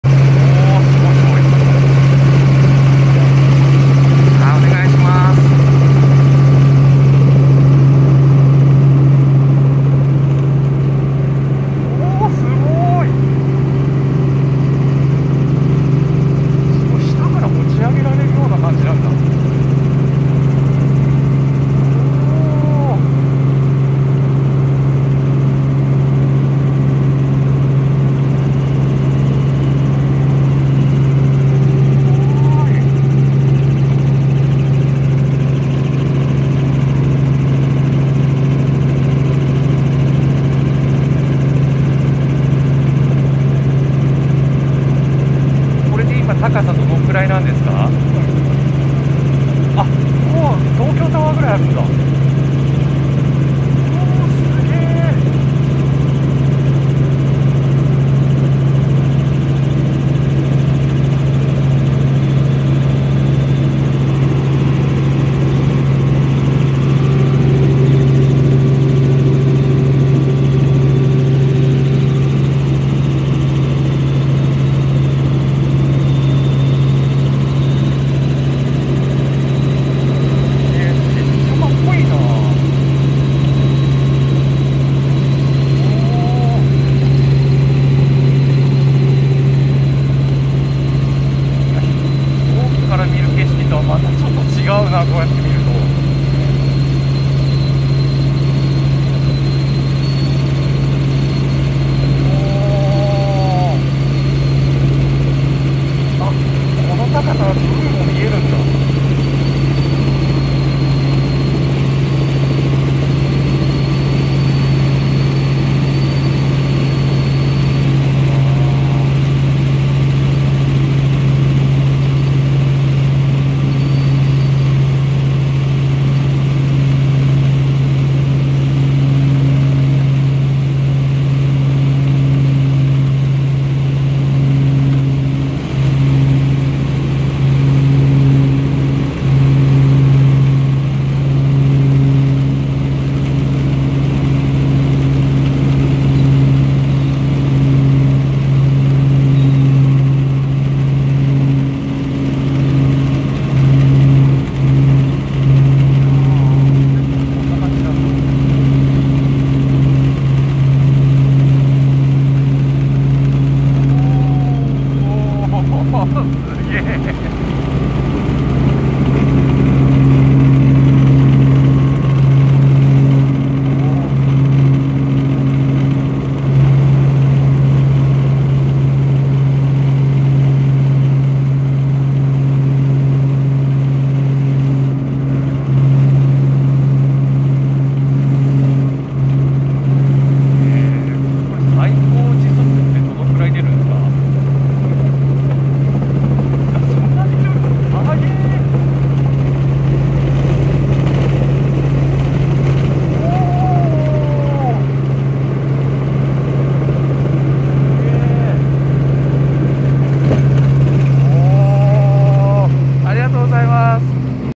御殿場プレミアム・アウトレット「ヘリクルージング」お試しコース3分の様子
2022年1月サービス開始の、御殿場プレミアム・アウトレット「ヘリクルージング」、お試しコース(フライト時間3分)の様子です。